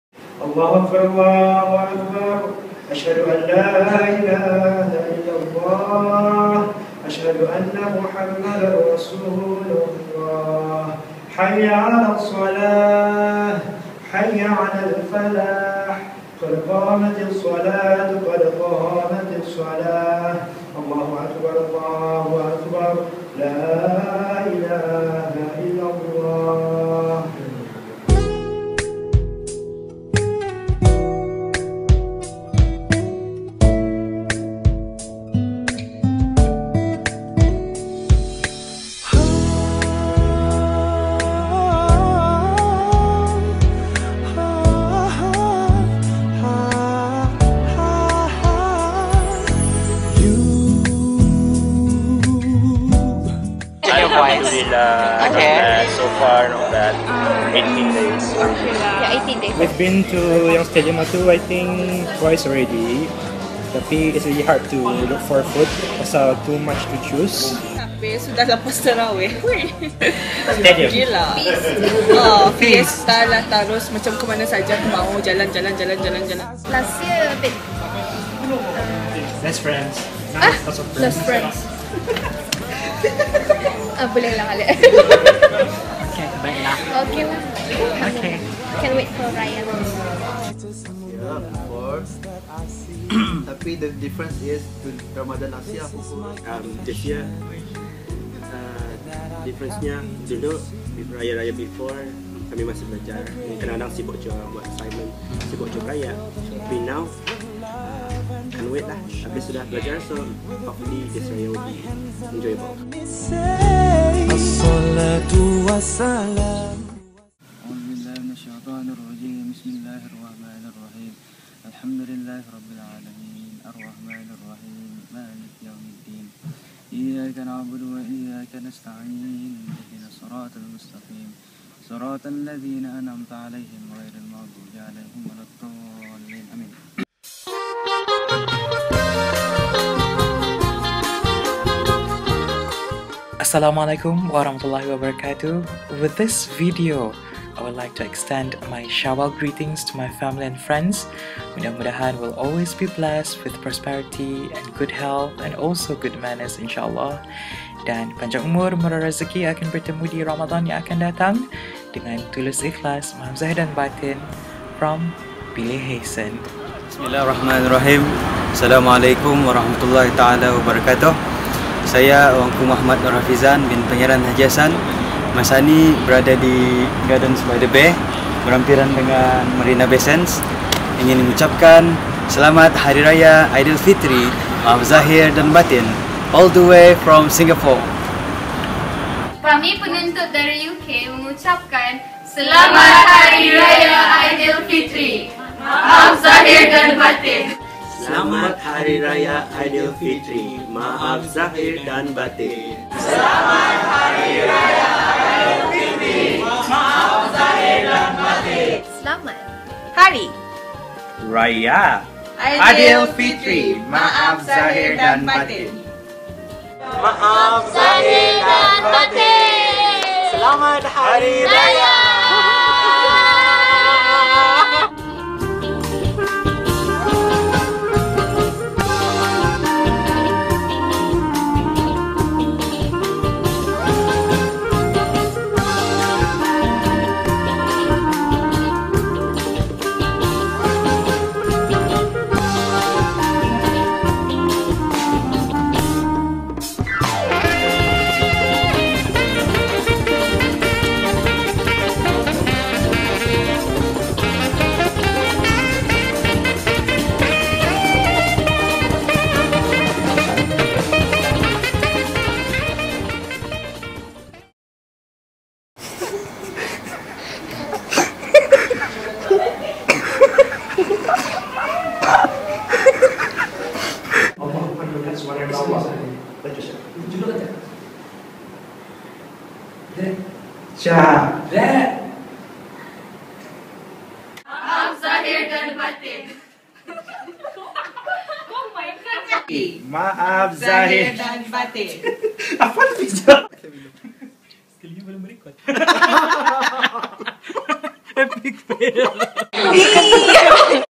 2013 - Raya Greetings!
Hi!

Apologies for the sound inconsistencies. I hope you can control the volume with your laptop/headset/vol. settings. Thanks to everyone who contributed their time and energy to the making of this short video! Selamat Hari Raya Aidilfitri Maaf Zahir dan Batin. Syawal 1434H, July 2013. Shot using my good ole' Sony camera (handheld) and ip5 (with personalised DIY tripod stand)